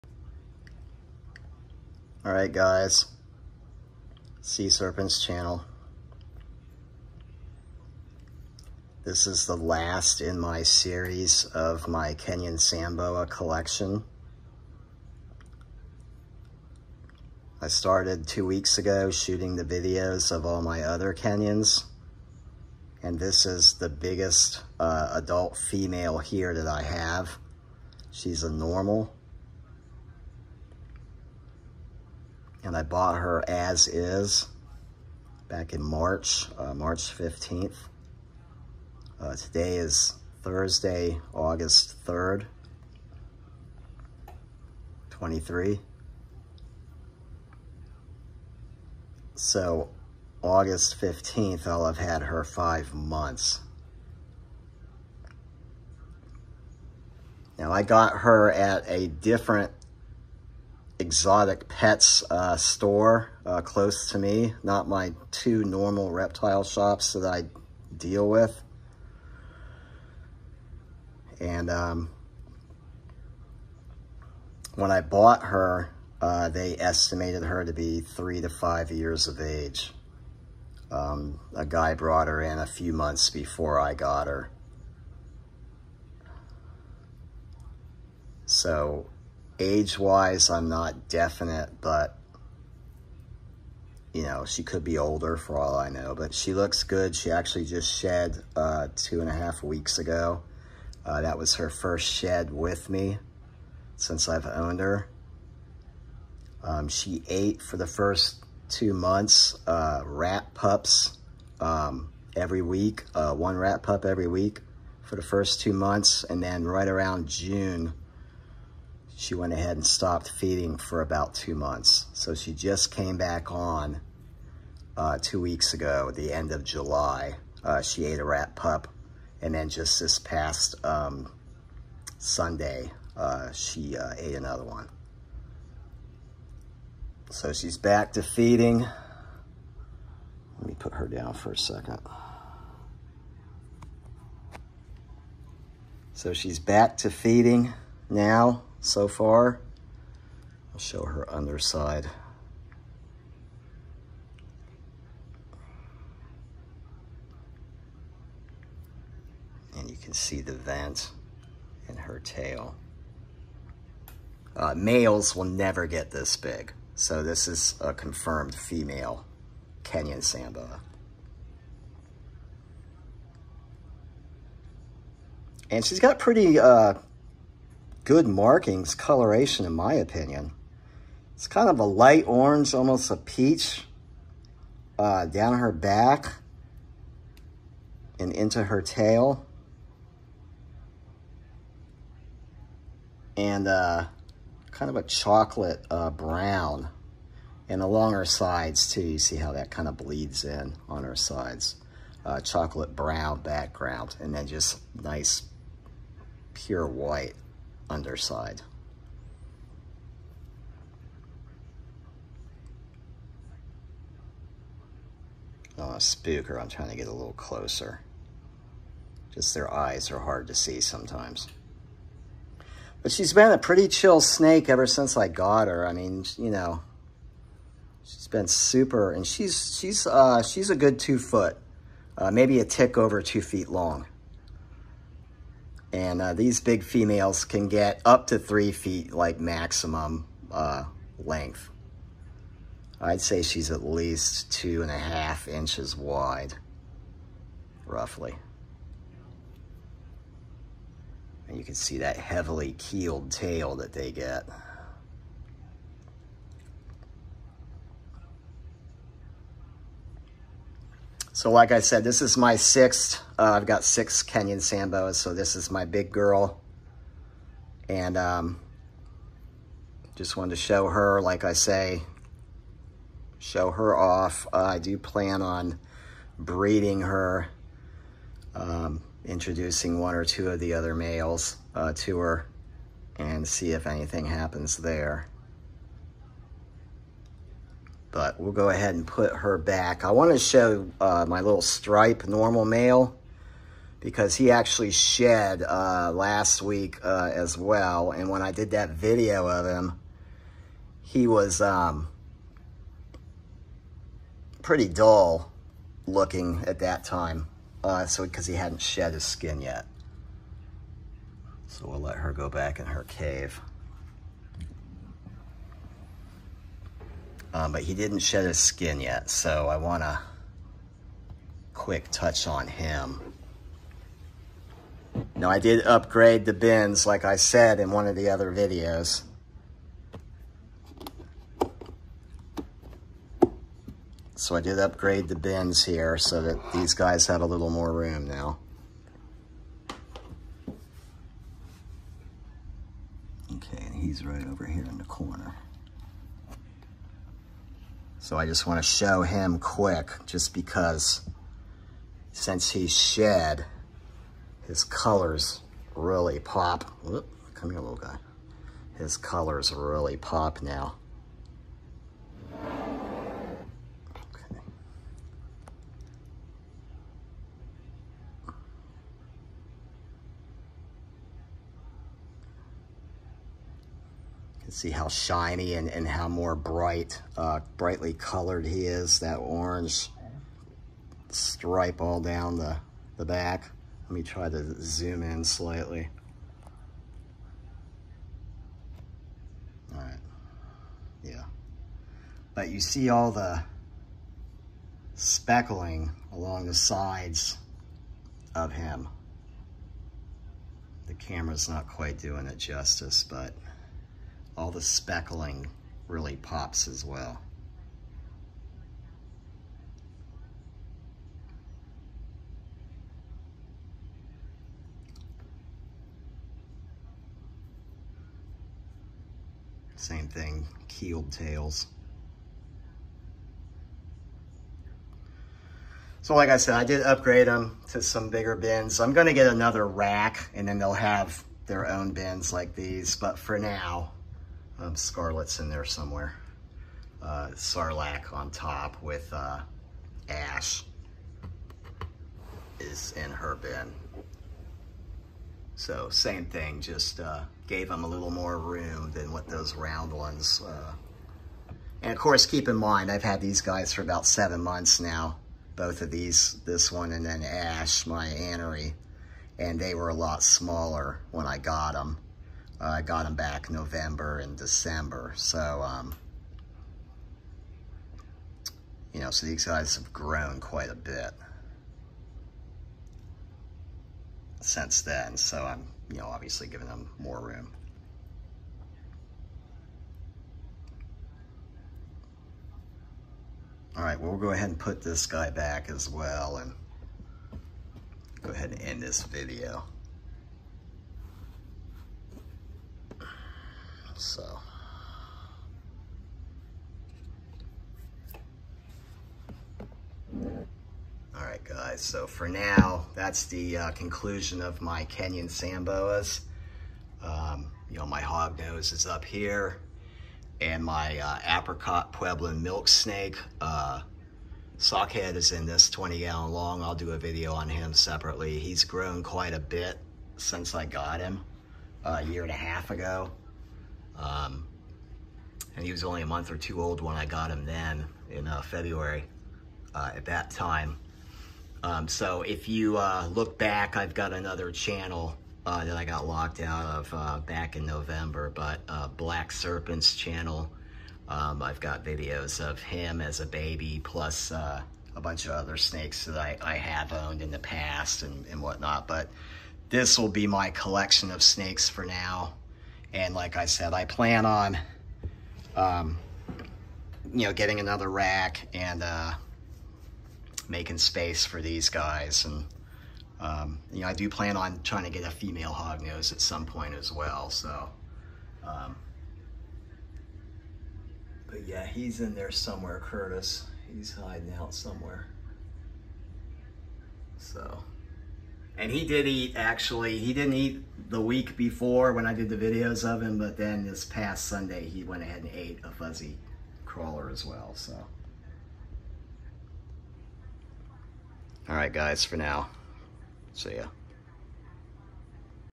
Kenyan sand boa collection:my big breeder size normal female #6
Showing my last Kenyan in the series of sand boas I have.